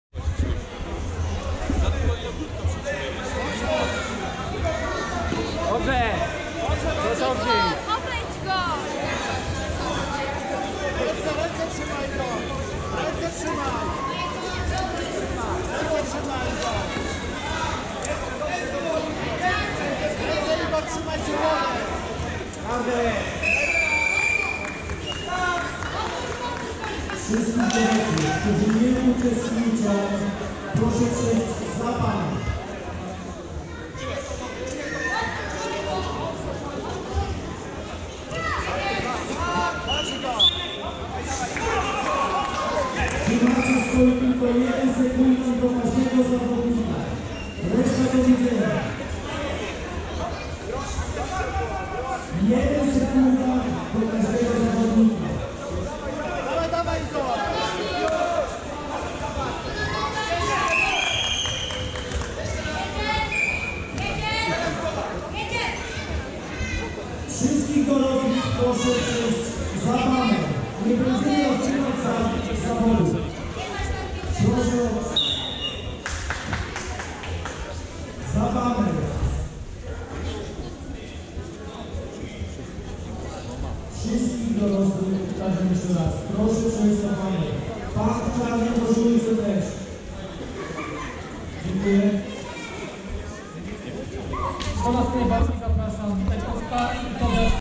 Igor Jarosz biały
Turniej Karate Kyokushin pod patronatem Prezydenta Miasta Będzina - Będzin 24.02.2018 r.